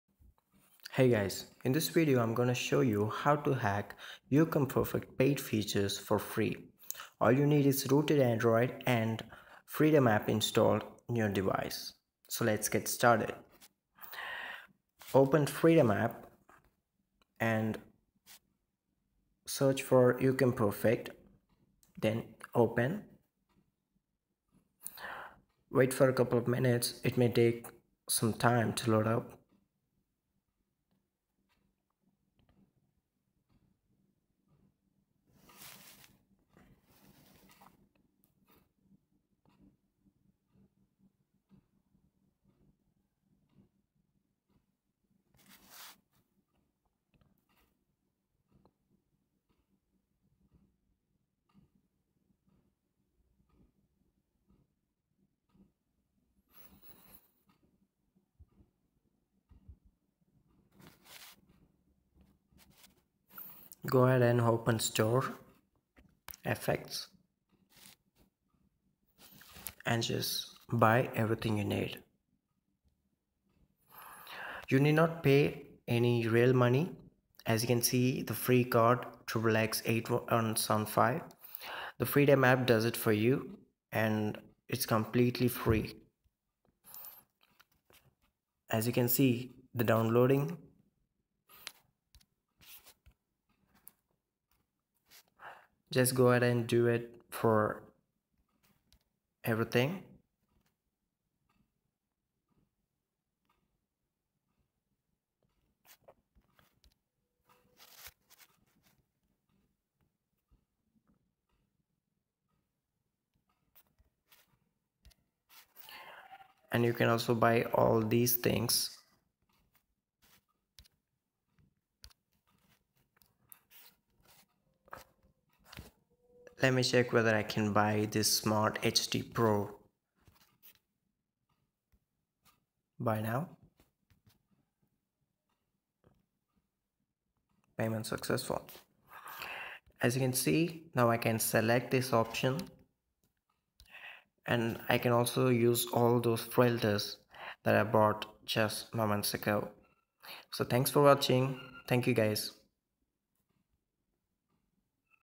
How to hack paid features in youcam perfect for free using in app purchase trick (root) (android)
How to hack or purchase paid features in youcam perfect for free using root access and freedom app